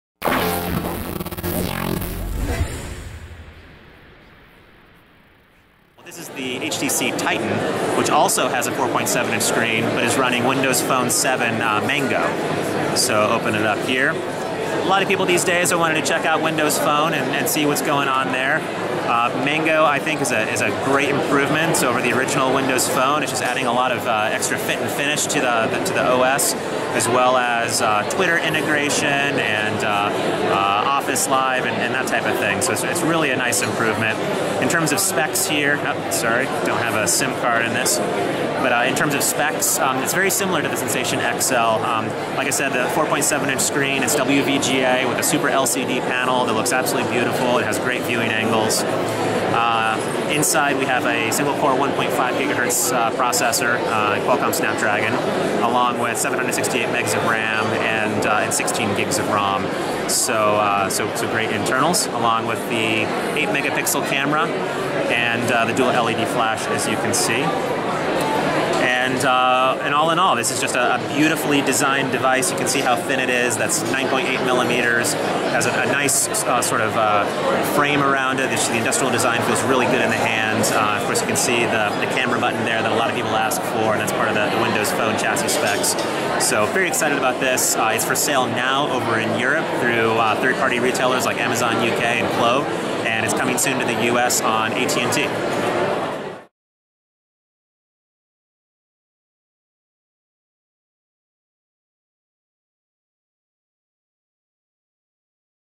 HTC Titan Hands-On - BWOne.com
The HTC Titian is a massive phone with a 4.7 inch Super LCD display. It comes with a 1.5 Ghz single-core snapdragon processor. 16 GB of built-in storage, 8 megapixel rear facing camera with 720p HD video capture,  1.3 megapixel front facing camera and is running Windows Phone 7.5 Mango the latest version of Microsoft smartphone operating system. The Titan is available now in the UK and will be coming to AT&T in the near future. Check out the hands on below.